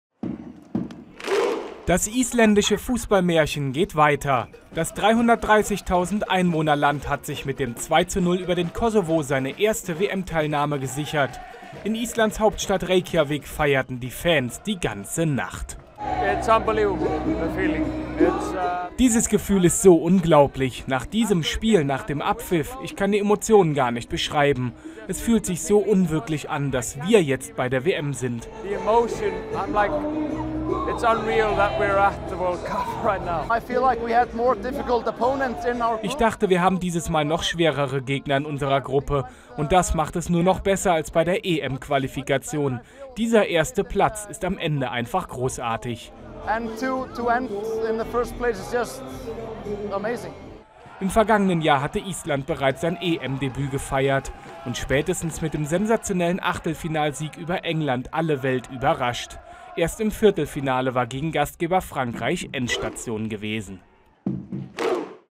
Fußballmärchen geht weiter: Island holt WM-Ticket
Die Nationalmannschaft Islands setzt ihr Fußballmärchen fort und hat sich zum ersten Mal für eine WM qualifiziert. Das Ticket wurde von den Fans in Reykjavik die ganze Nacht gefeiert.